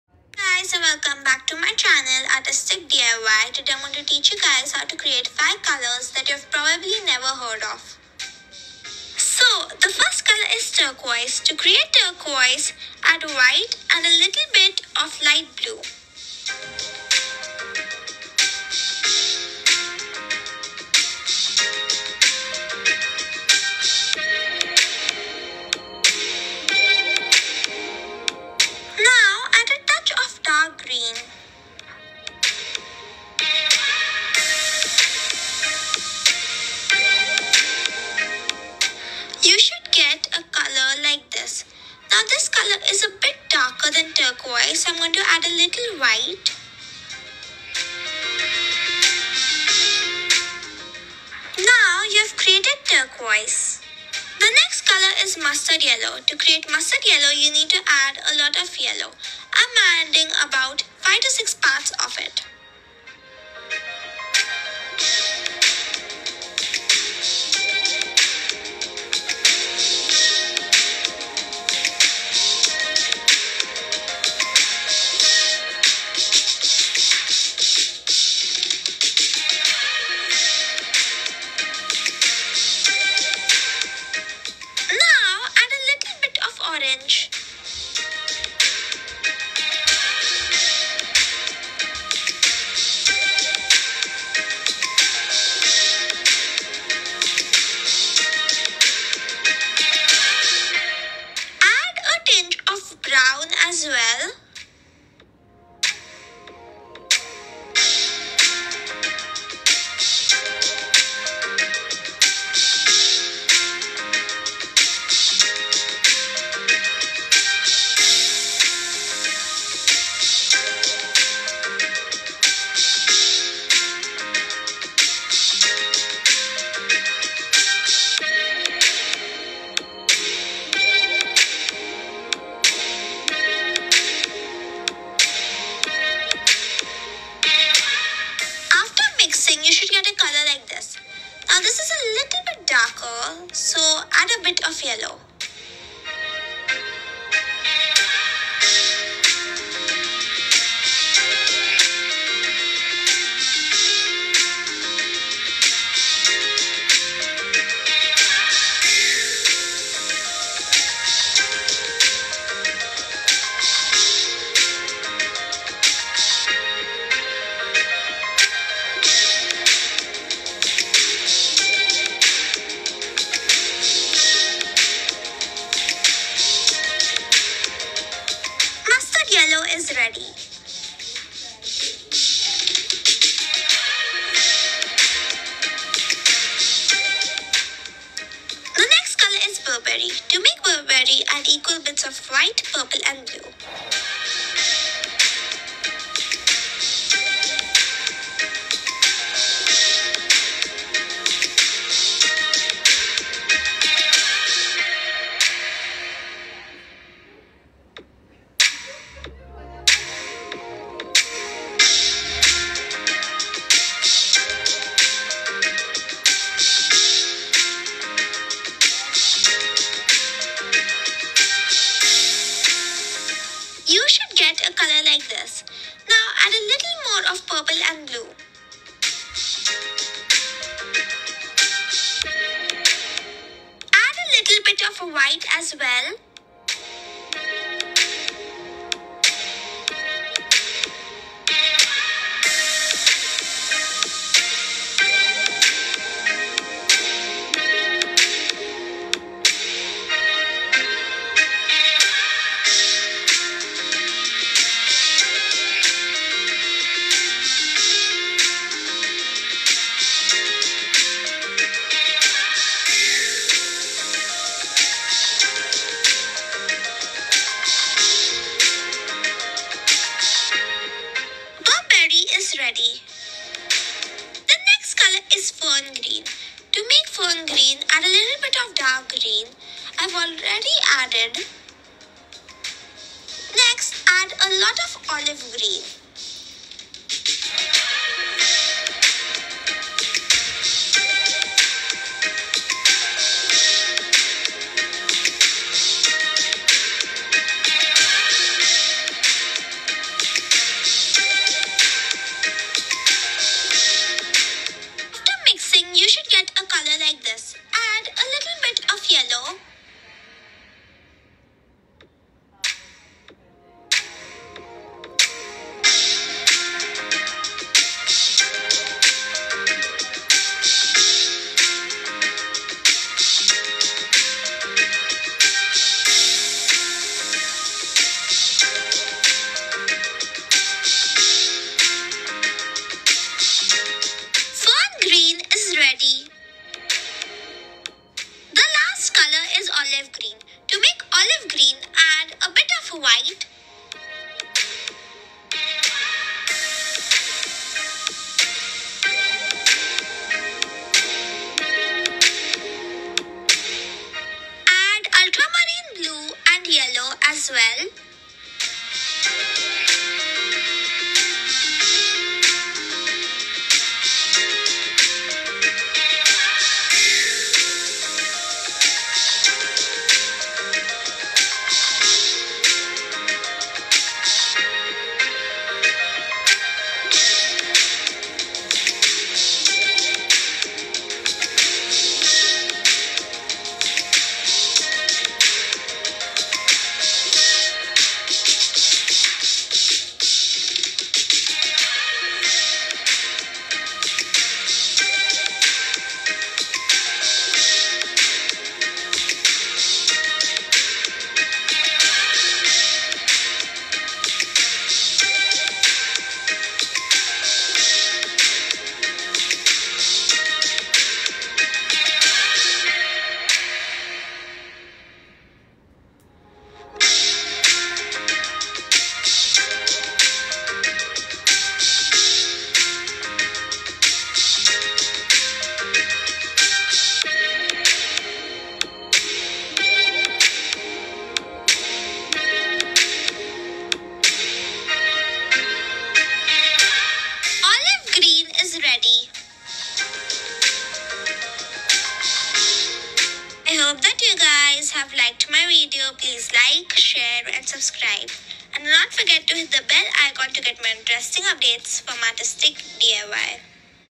How to make colours that you have probably never heard of|blueberry, mustard yellow,fern green, etc
in this video we show you how to make colours like blueberry, turquoise, mustard yellow, fern green and olive green.